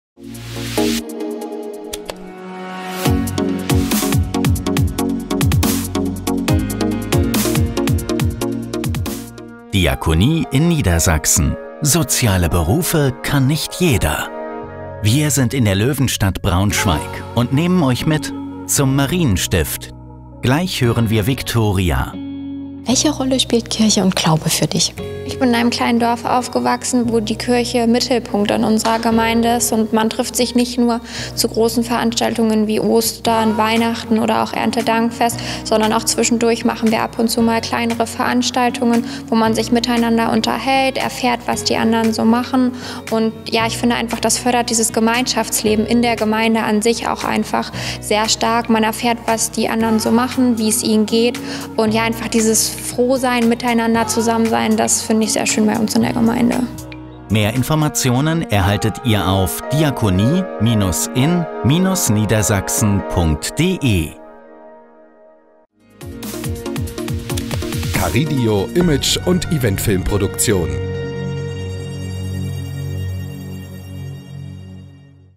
Mein FSJ bei der Diakonie Braunschweig mit Viktoria - Freiwilliges soziales Jahr - Trailer ohne UT
Mein FSJ bei der Diakonie Braunschweig mit Viktoria - Freiwilliges soziales Jahr - Trailer ohne Untertitel

Was ist ein Freiwilliges Soziales Jahr?

Während eines Freiwilligen Sozialen Jahres (FSJ) engagieren sich junge Menschen in sozialen Einrichtungen wie zum Beispiel Krankenhäusern, Kitas oder Seniorenheimen. Ein FSJ geht in der Regel ein Jahr, ist generell aber zwischen sechs und 18 Monaten möglich. Wer ein FSJ machen möchte, muss zwischen 15 und  27 Jahren sein und die allgemeine Schulpflicht erfüllt haben. Im Rahmen von Projekten gibt es für Gruppen oder mehrere Zielpersonen bei einzelnen Trägern die Möglichkeit das  auf bis zu 24 Monate auszudehnen. Ein FSJ ist eine Bildungs- und Orientierungszeit und ermöglicht Jugendlichen und jungen Erwachsenen Erfahrungen in sozialer Arbeit zu sammeln. Das FSJ in der Diakonie gibt es seit April 1954 – gesetzlich geregelt ist es seit 1964. Über die Tätigkeit in einer Einsatzstelle hinaus haben alle Freiwilligendienstleistenden eine pädagogische Begleitung. Dieses Programm schließt 25 Seminartage ein. Die Inhalte orientieren sich auch an den Interessen und Bedürfnissen der Freiwilligen. Ein FSJ, das bei evangelischen Trägern statt findet, heißt auch Diakonisches Jahr (DJ) oder FSJ in der Diakonie. Ein Diakonisches Jahr im Ausland (DJiA) wird ebenfalls von evangelischen Trägern betreut und gleicht dem FSJ, wird aber im Ausland absolviert (“Outgoing“). Entsprechend gibt es für junge Menschen aus anderen Herkunftsländern auch die Möglichkeit, ein FSJ in Deutschland zu machen (“Incoming“).
Das in den 90er Jahren entstandene und 1993 gesetzlich geregelte Freiwillige Ökologische Jahr (FÖJ) ist auch wie das klassische FSJ aufgebaut – allerdings mit einem ökologischen Schwerpunkt.

Eine Produktion von Karrideo Image- und Eventfilmproduktion, Inh. Christian Weiße, Johann-Degen-Straße 13, 51105 Köln